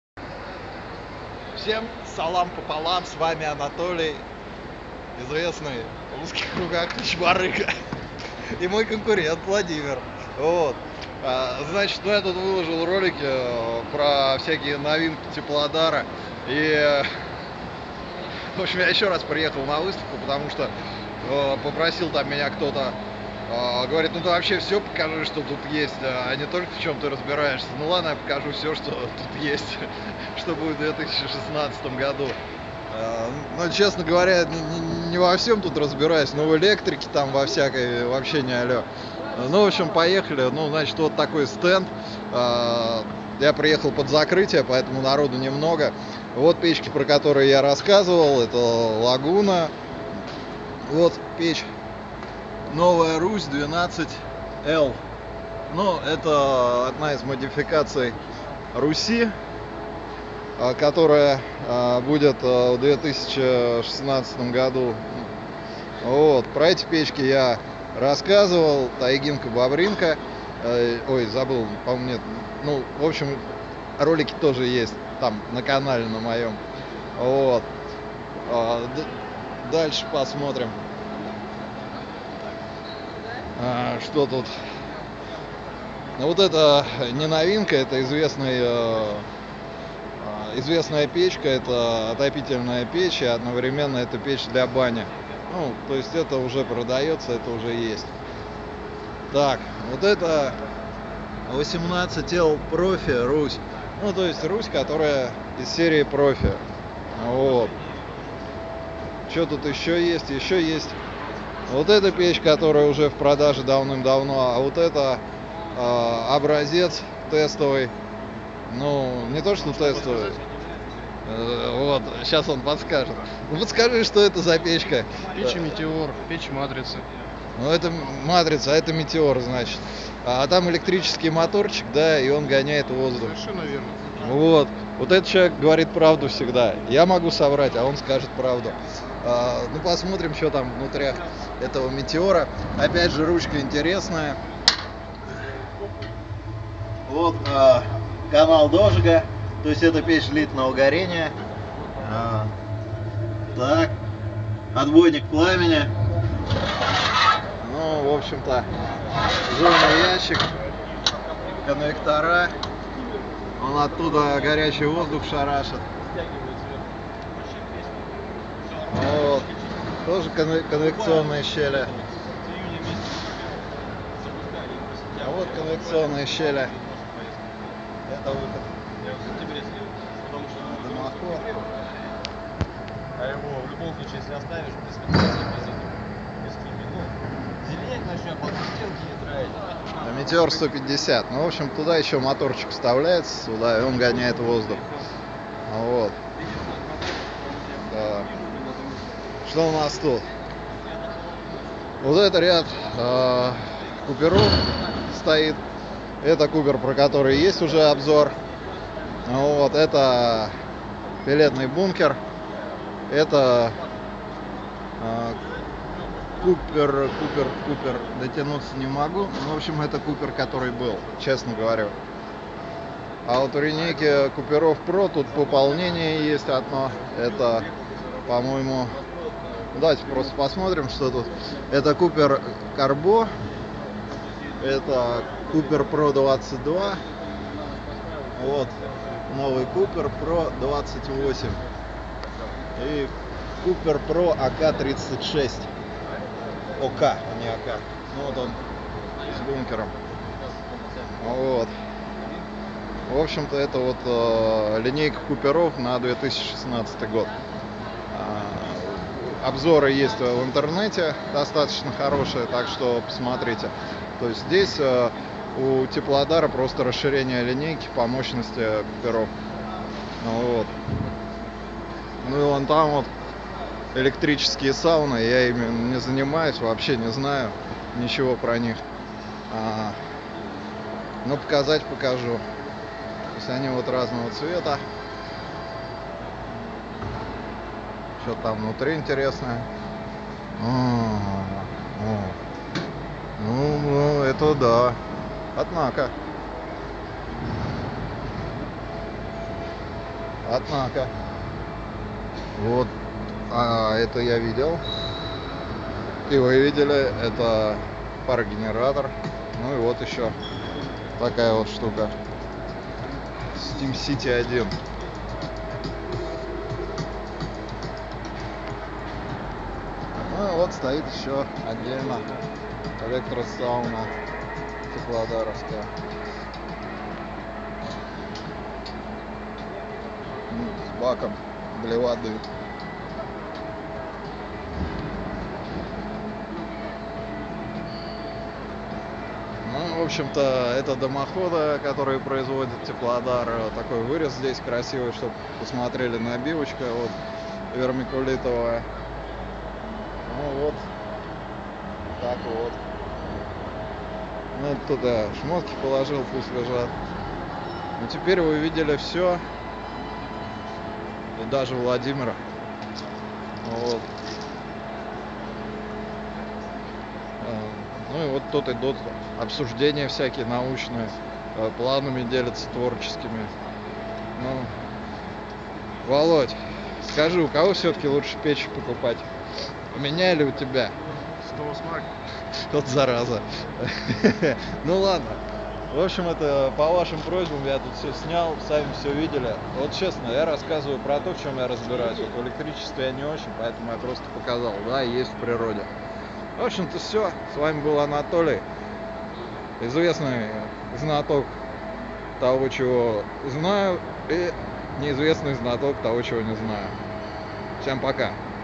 Что привез Теплодар на выставку Акватерм 2016 ?
Коротенький забег по стенду с включенным телефоном